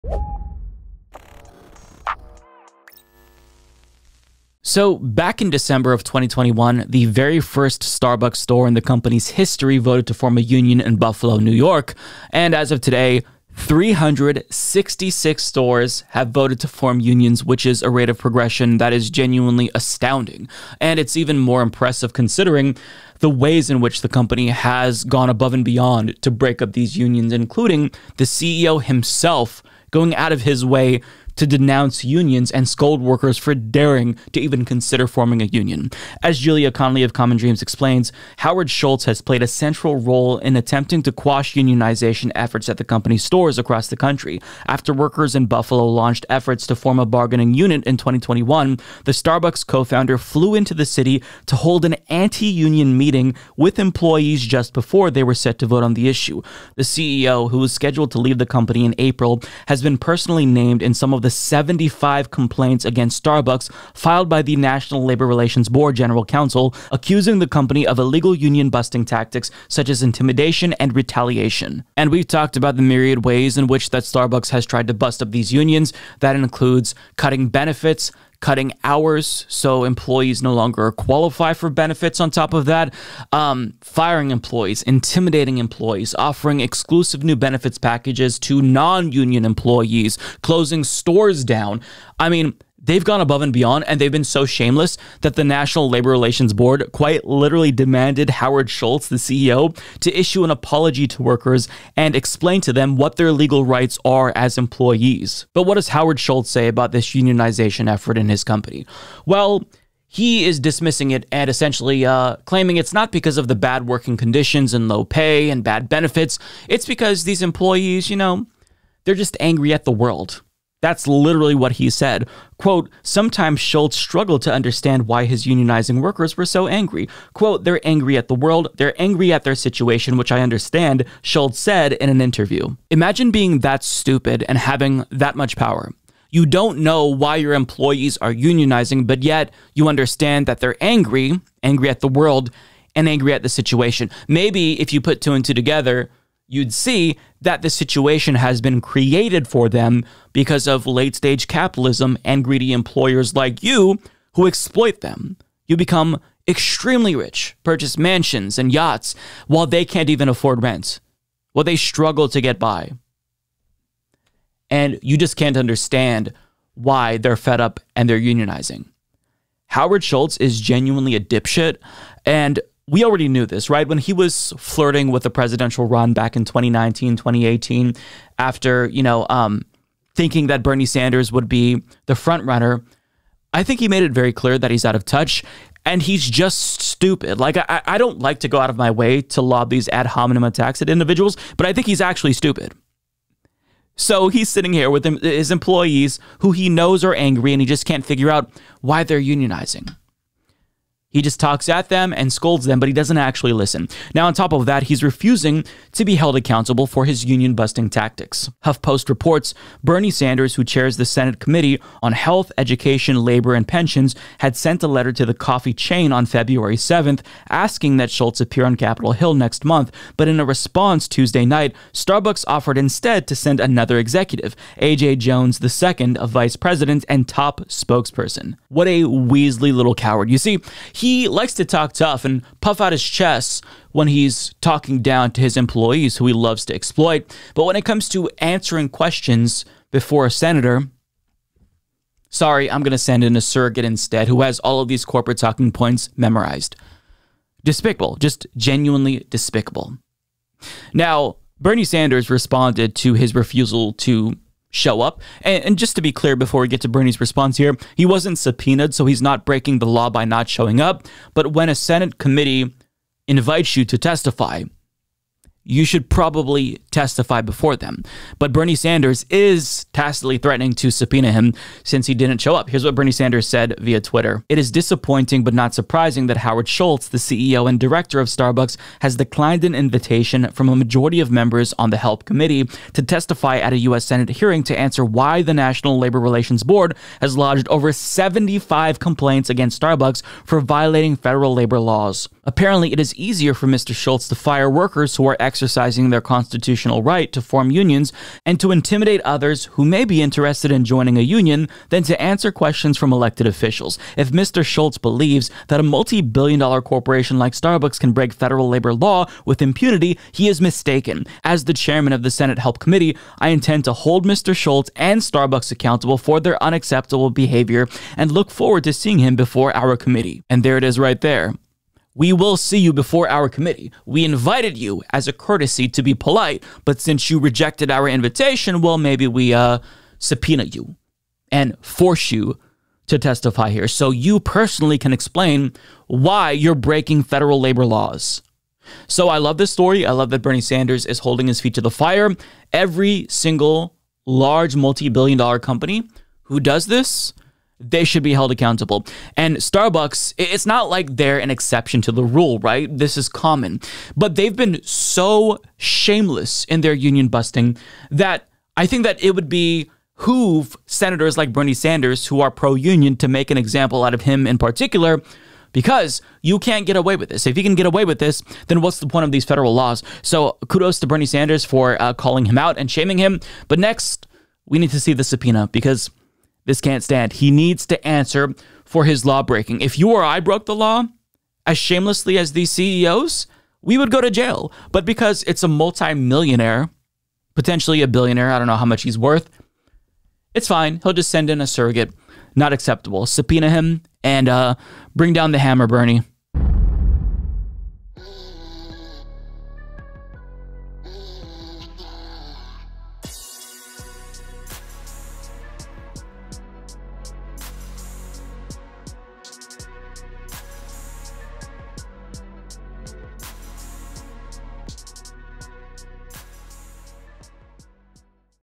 Bernie Sanders Slams Starbucks’ Union-Busting CEO for Refusing to Testify Before Senate Committee
Starbucks' CEO Howard Schultz declined an invitation to testify before the Senate Committee on Health, Education, Labor and Pensions. In response the Chair of that committee, Bernie Sanders, wasn't happy. Sanders put out a scathing statement denouncing Schultz and tacitly threatening him with a subpoena.

Chapters:
0:00 Howard Schultz Busting Unions
1:45 Schultz on Why Stores Unionize
3:24 Bernie Sanders Goes After Schultz
6:37 Outro